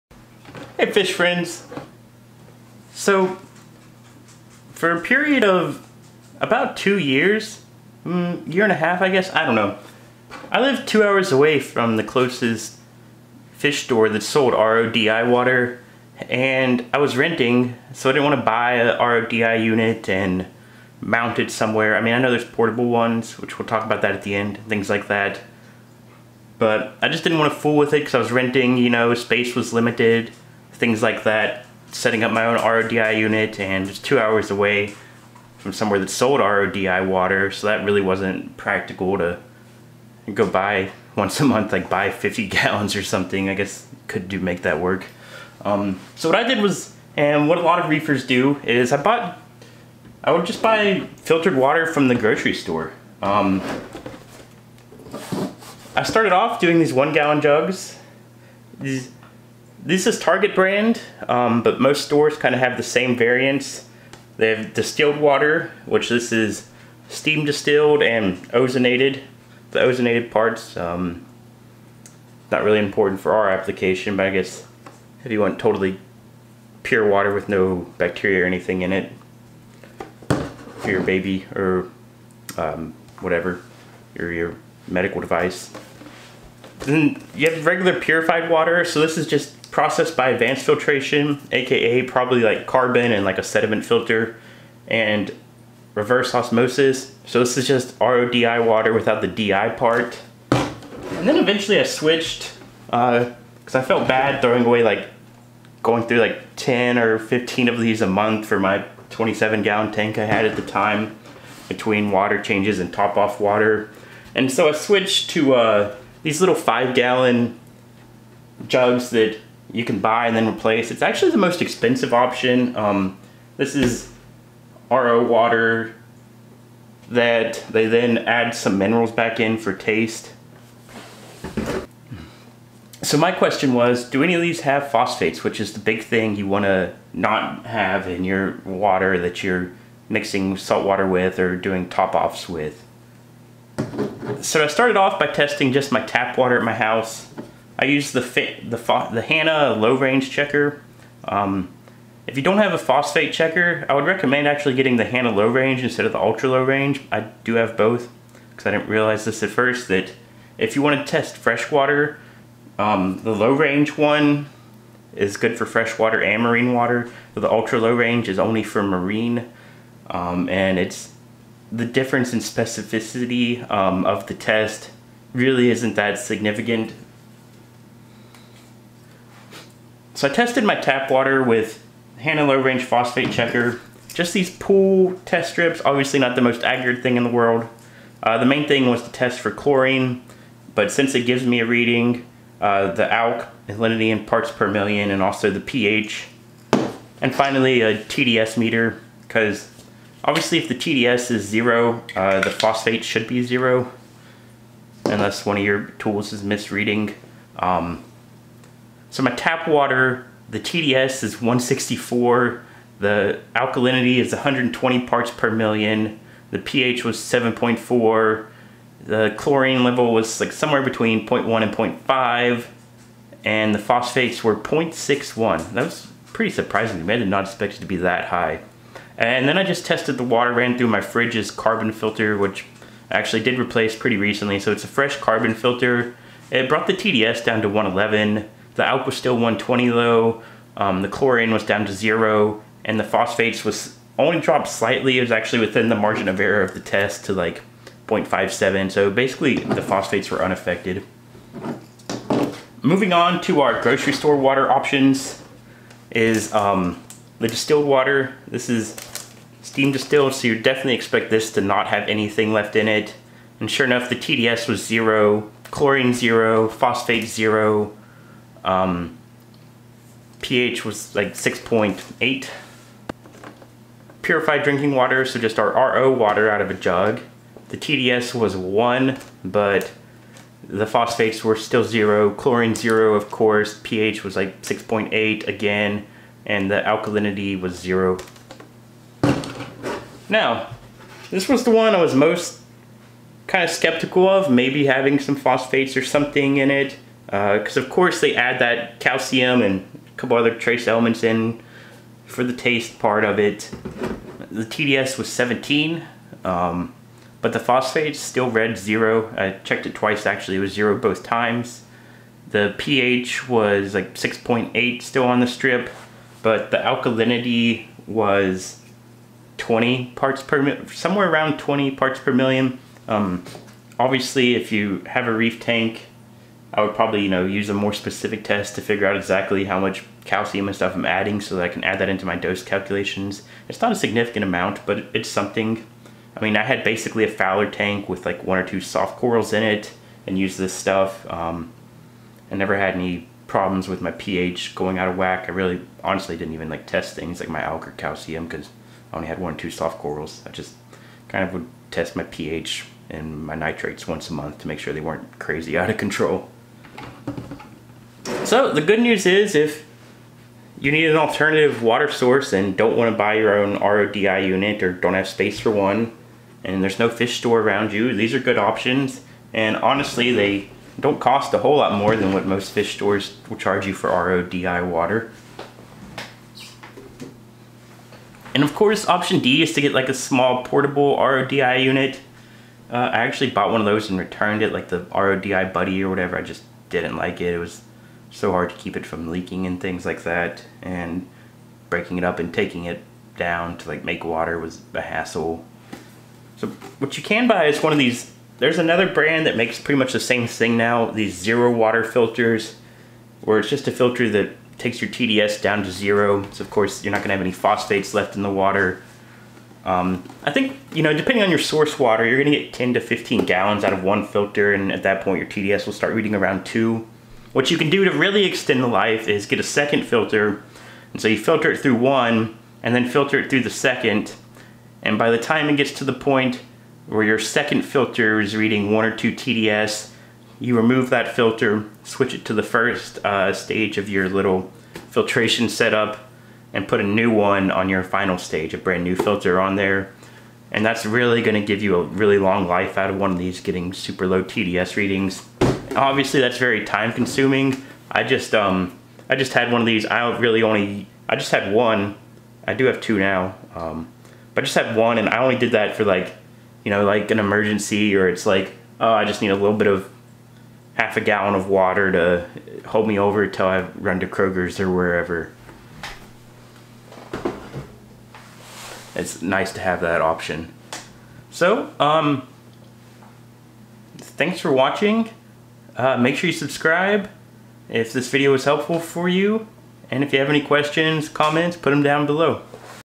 Can you use grocery store water in your reef tank?
Can you use bottled grocery store water in your home reef tank? Or best left for fowlr tank only?

- Amazon link to check out zero water filters
Bed Bath and Beyond also sells these in stores normally, if you stack with a coupon can get a pretty good deal